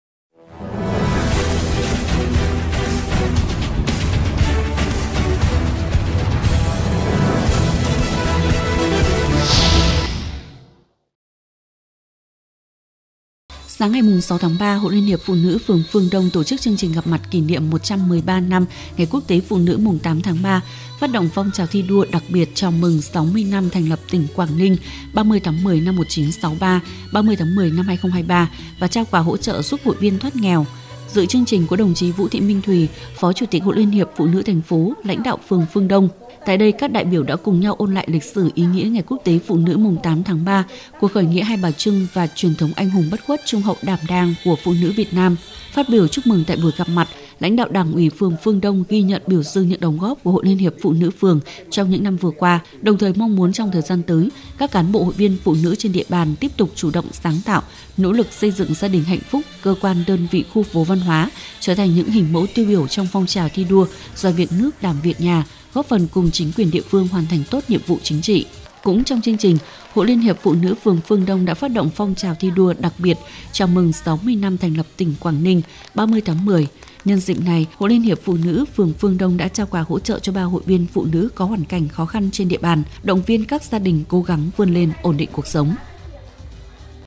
Gặp mặt kỷ niệm 113 năm Ngày Quốc tế phụ nữ 8/3
Sáng 6/3, Hội LHPN phường Phương Đông tổ chức chương trình Gặp mặt kỷ niệm 113 năm Ngày Quốc tế phụ nữ 8/3, phát động phong trào thi đua đặc biệt chào mừng 60 năm thành lập tỉnh Quảng Ninh 30/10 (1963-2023) và trao quà hỗ trợ giúp hội viên thoát nghèo. Dự chương trình có đồng chí Vũ Thị Minh Thùy, Phó Chủ tịch Hội LHPN thành phố; lãnh đạo phường Phương Đông.